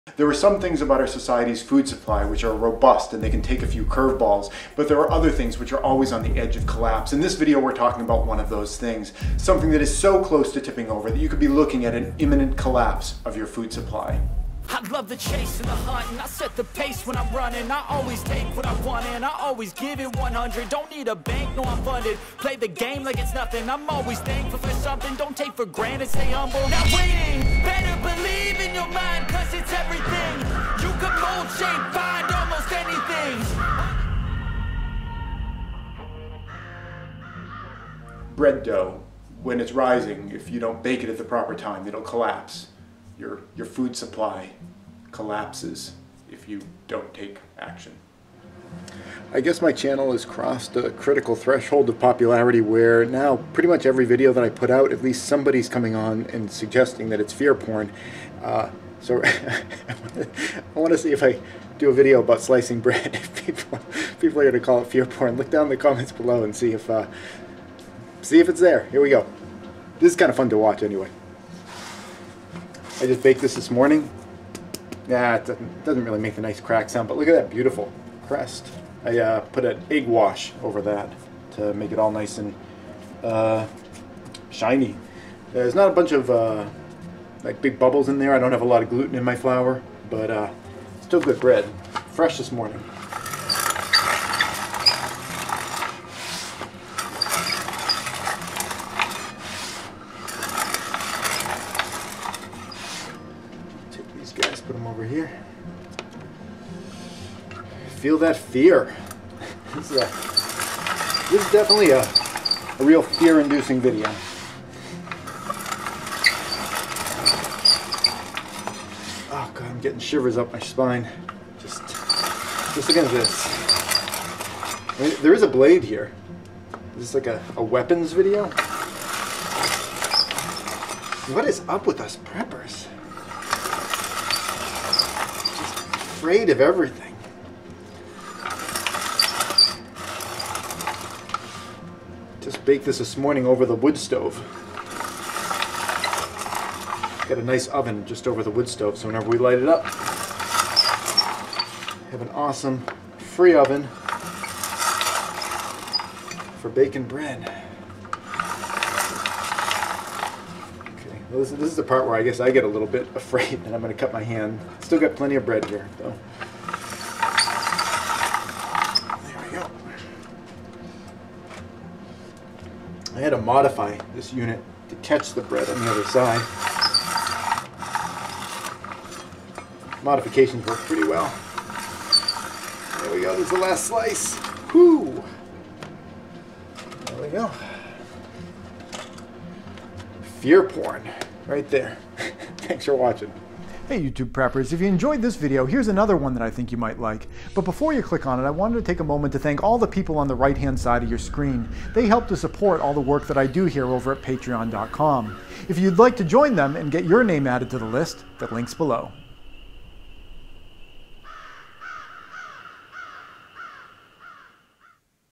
RED ALERT: Total Collapse within 24 Hours (without action)
Without intervention this food supply will collapse within 24 hours! Emergency Alert for the USA and for preppers. Get ready for total collapse!

ITEMS MENTIONED IN THIS VIDEO:
Zassenhaus Bread Slicer